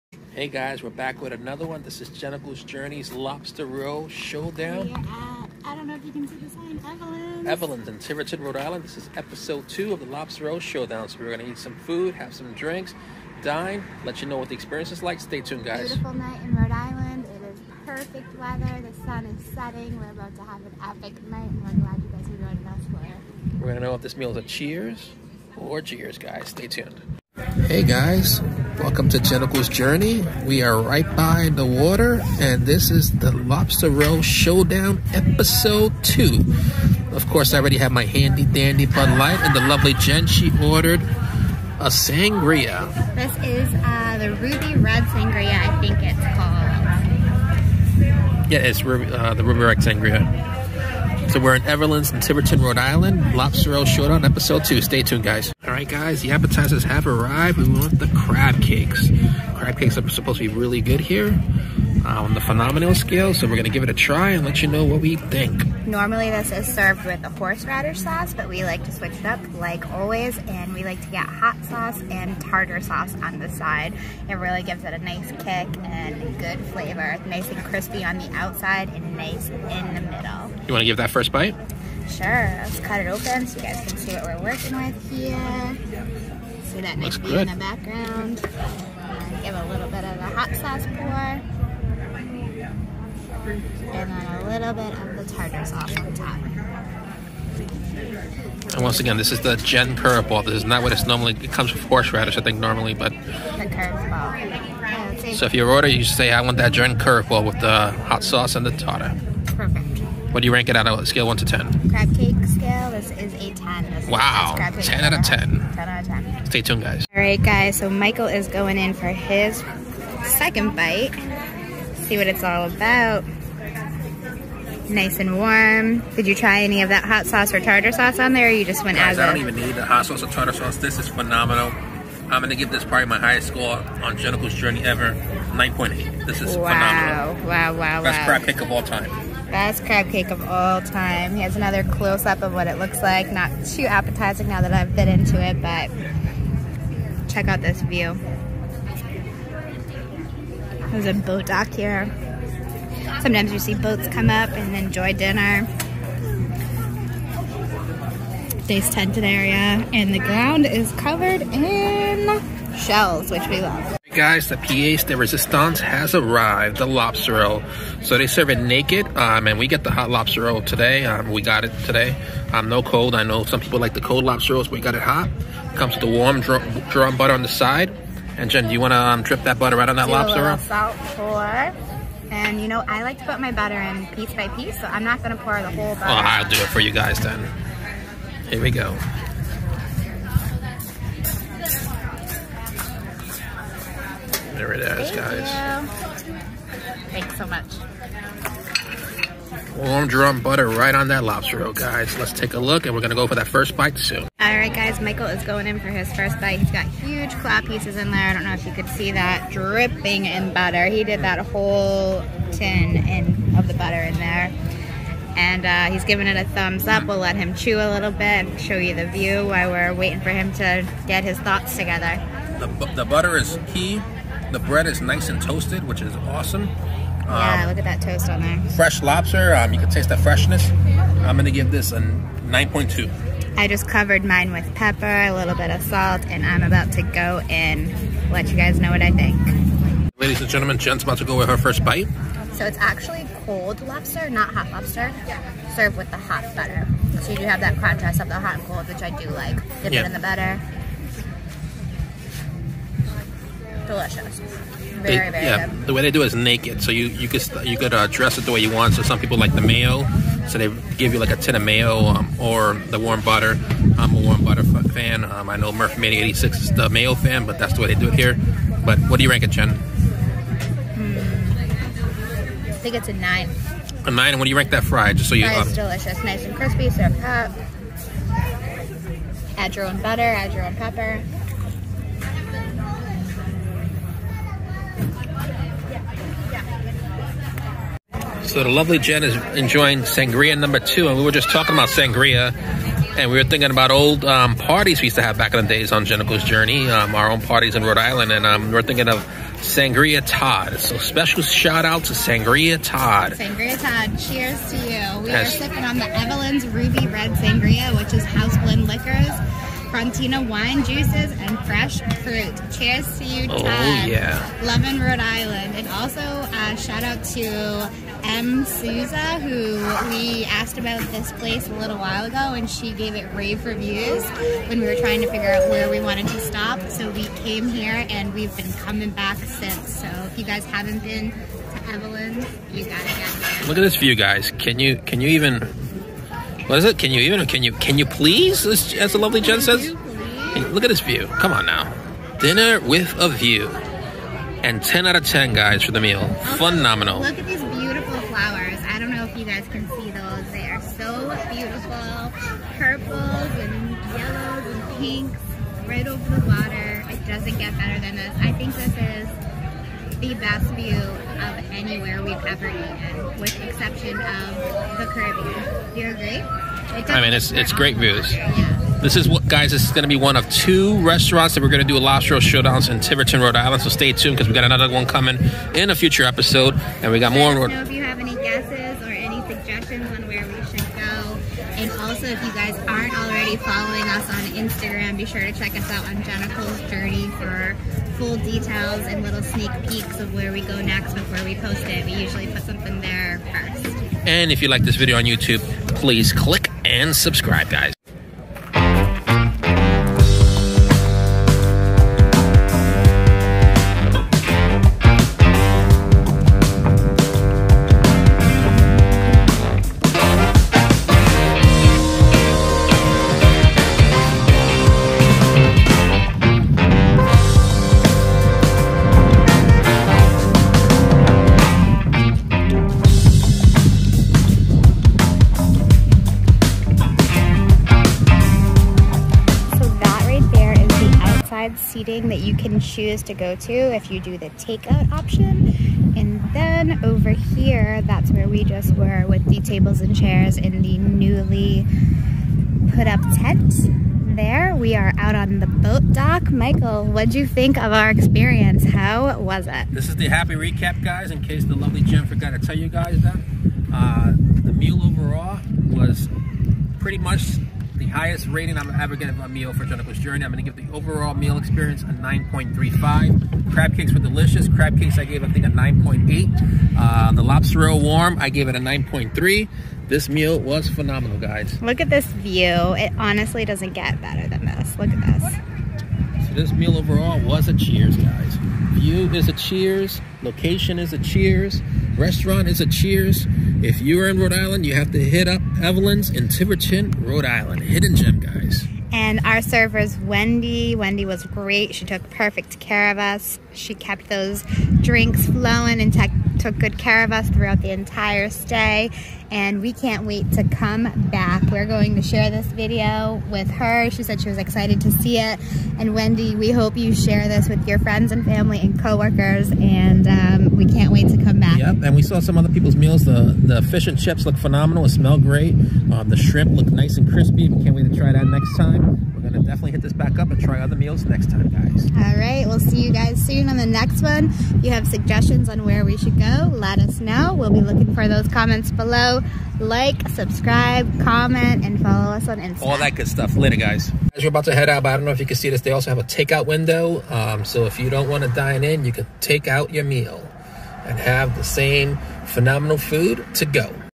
Evelyn's Drive-In, as seen on Good Morning America, Tiverton, RI Lobster Roll Showdown Episode 2 🦞
Featured on Good Morning America 7/26/2021

Waterside clam shack known for its fried seafood with indoor / outdoor seating & takeaway, since 1969.

Best restaurant view in Rhode Island

Sampled:
•Crab Cakes
•Lobster Roll with warm butter & fries x2
•Evelyn's Ruby Red Sangria

📍Location
2335 Main Road
Tiverton, RI

Check out our Zero Gravity beer review we did earlier down the street at Fogland Beach

& Follow our journey on Instagram @JenichaelsJourney